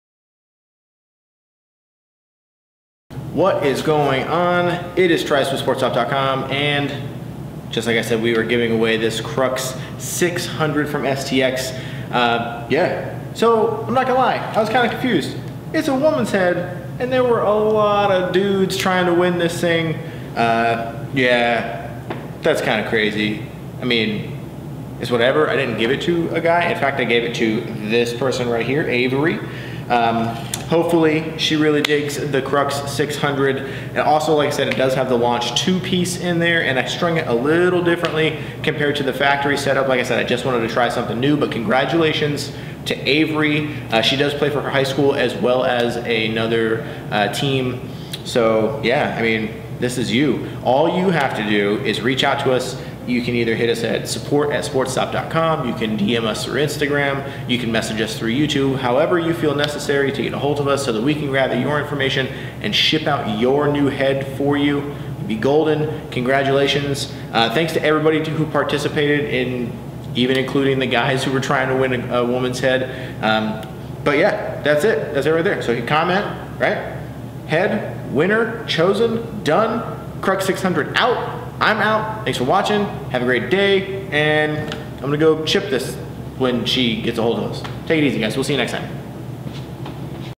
STX Crux 600 Women's Lacrosse Head Giveaway Winner Announcement Video
Congratulations to Avery for winning the STX Crux 600 Women's Lacrosse Head strung with a STX Launch 2 Pocket Piece.